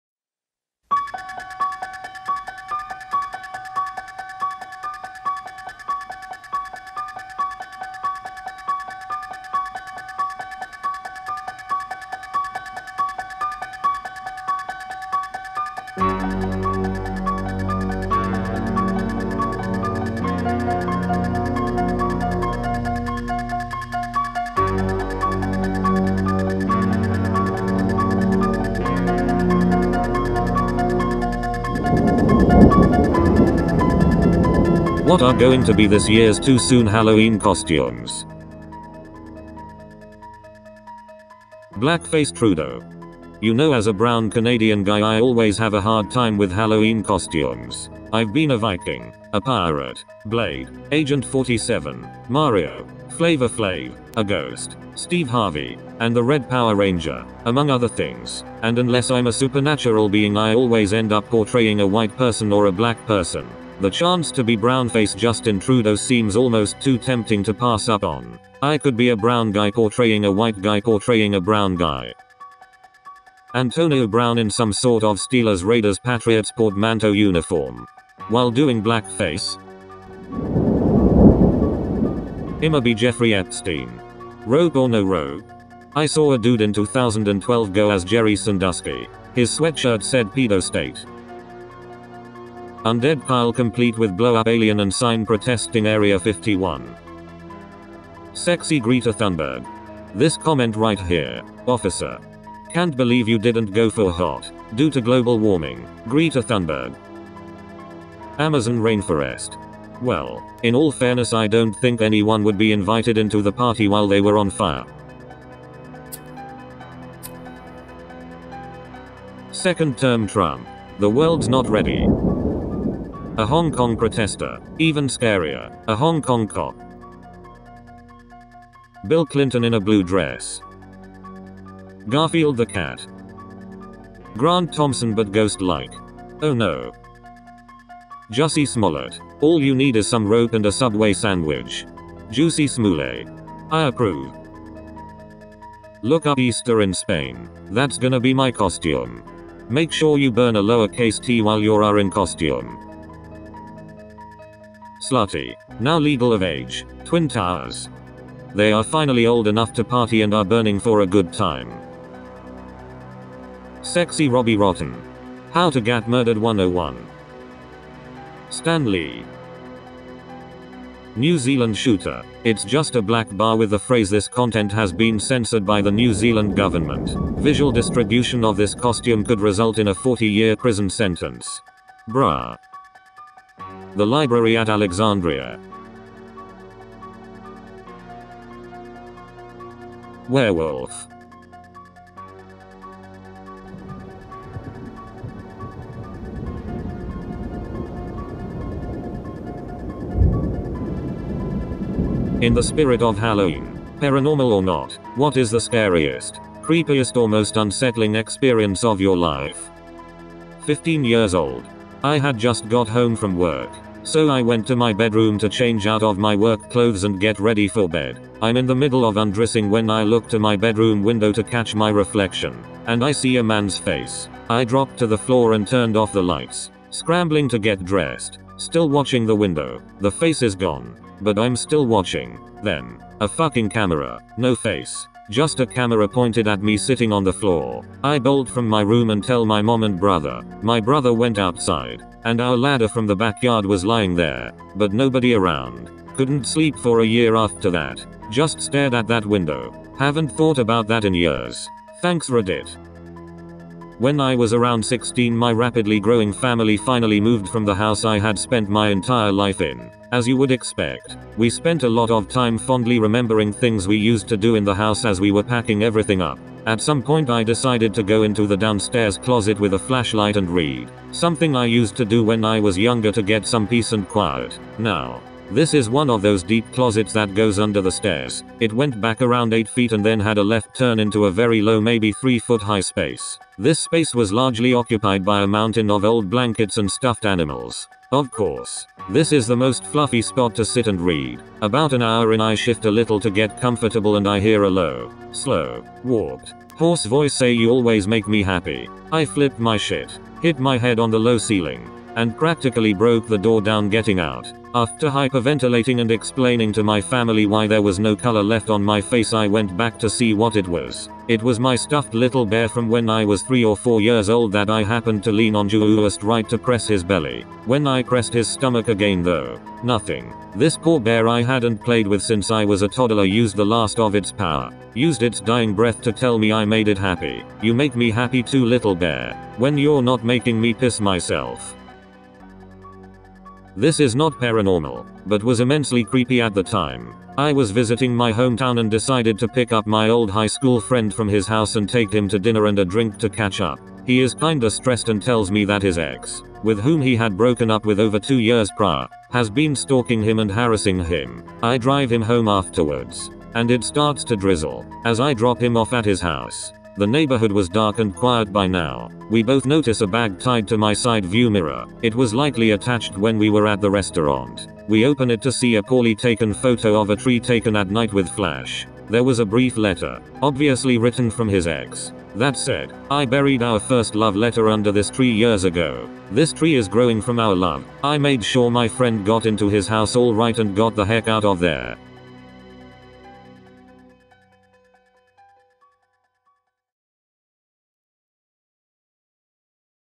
What's this years "too soon" halloween costume? Reddit Halloween Compilation 2019
Halloween is coming and this reddit compilation is just what you need to get you in the mood!

Comment your costume ideas down below 👇

I tried to improve the video by increasing the text size so it looks better on smaller screens and new paragraphs appear in a pumpkin orange. I hope it makes it easier to follow along.